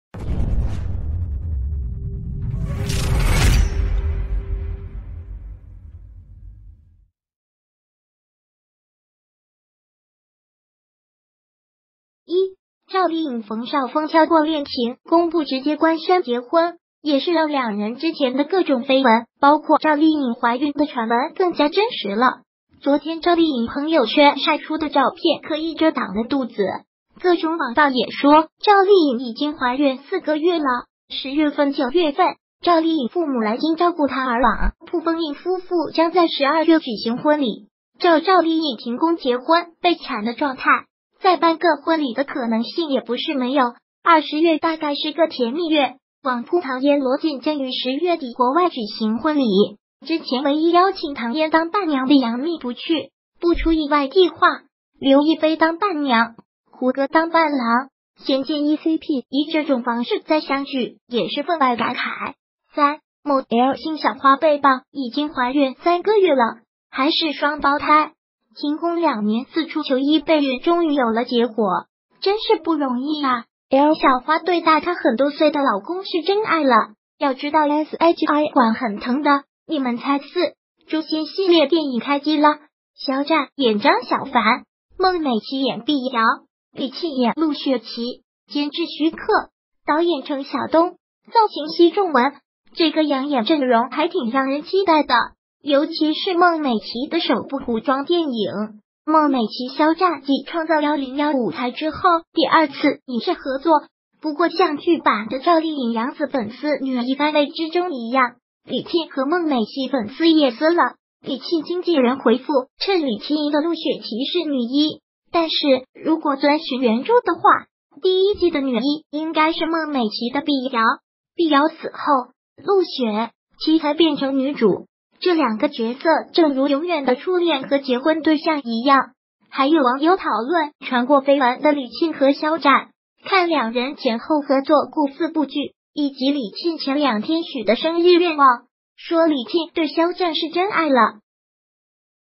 赵丽颖怀孕4月，唐嫣罗晋婚礼伴郎伴娘？《诛仙》肖战孟美岐李沁
1、赵丽颖冯绍峰跳过恋情公布直接官宣结婚，也是让两人之前的各种绯闻包括赵丽颖怀孕的传闻更加真实了。昨天赵丽颖朋友圈晒出的照片刻意遮挡了肚子，各种网曝也说赵丽颖已经怀孕4个月了，10月份9月份赵丽颖父母来京照顾她而网曝峰颖夫妇将在12月举行婚礼，照赵丽颖停工结婚备产的状态，再办个婚礼的可能性也不是没有。2、10月大概是个甜蜜月，网曝唐嫣罗晋将于10月底国外举行婚礼，之前唯一邀请唐嫣当伴娘的杨幂不去，不出意外地话，刘亦菲当伴娘，胡歌当伴郎。仙剑一cp以这种方式再相聚也是分外感慨。3、某L姓小花被曝已经怀孕三个月了，还是双胞胎。停工两年四处求医备孕终于有了结果，真是不容易啊。L小花对大她很多岁的老公是真爱了，要知道shi管很疼的~（你们猜）4、《诛仙》系列电影开机了，肖战演张小凡，孟美岐演碧瑶，李沁演陆雪琪。监制徐克，导演程小东，造型奚仲文。这个养眼阵容还挺让人期待的，尤其是孟美岐的首部古装电影，孟美岐肖战继《创造101》舞台之后第二次影视合作。不过像剧版的赵丽颖杨紫粉丝女一番位之争一样，李沁和孟美岐粉丝也撕了，李沁经纪人回复称李沁的陆雪琪是女一。但是如果遵循原著的话，第一季的女一应该是孟美